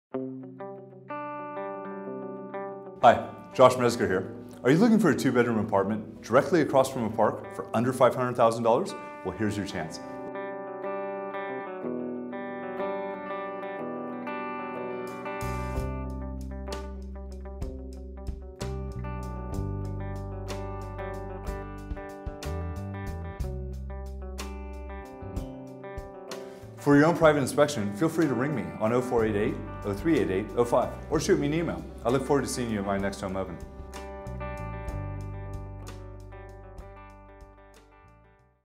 Innercity Simplicity by Josh Mezger
Are you looking for a 2 Bedroom, 1 Bathroom, apartment in the popular Parkview Perth Apartments in Perth? You will not find better value in the current market as apartments of this size, aspect and scope are rarely for sale.

The many amenities include, but are not limited to:

Top floor, North facing apartment
Directly across from Wellington Square Park
Yellow Cat bus line directly in front
Hop, skip, and a jump to the Perth Train Station, Northbridge Precinct & WACA
Open plan living with reverse cycle air conditioning
Rental potential of $500 / week U/F and $550 / week F/F
Built in robes to both bedrooms
Undercover parking available for one car
2 x large balconies, both north and south facing
Large, chef's kitchen

Council Rates: Approximately $1500.00 p.a.
Water Rates:Approximately $1080.00 p.a.
Strata Levies: Approximately $2600.00 p.a.